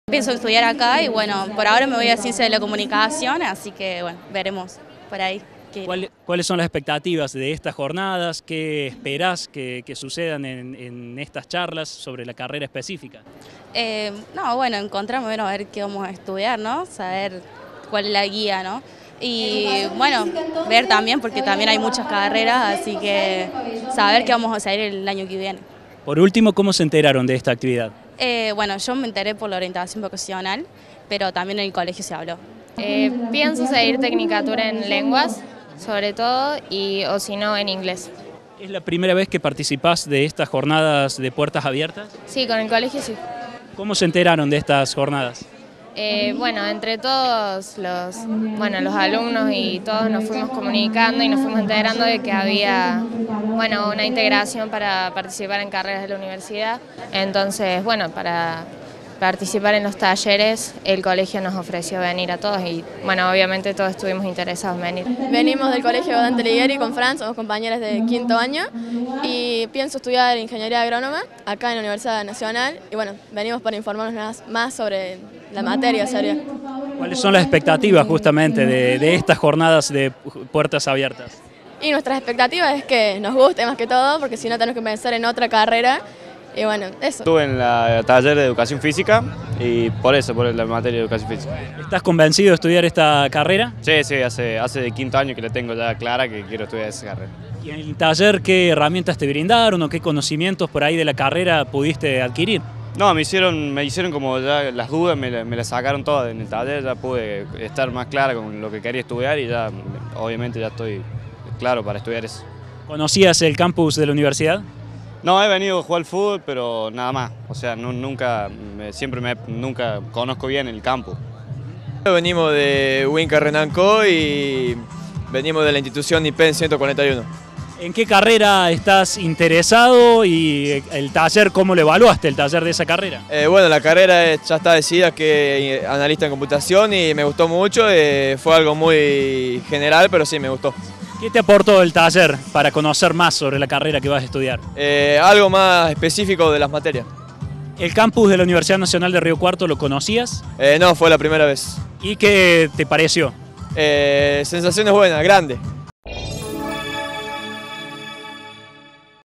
UNIVERSIDAD DE PUERTAS ABIERTAS - ESTUDIANTES
CIENTOS DE ESTUDIANTES SECUNDARIOS COLMARON EL CAMPUS EN EL INICIO DE LAS JUPA
Facebook.com/uniriotv
Twitter.com/uniriotv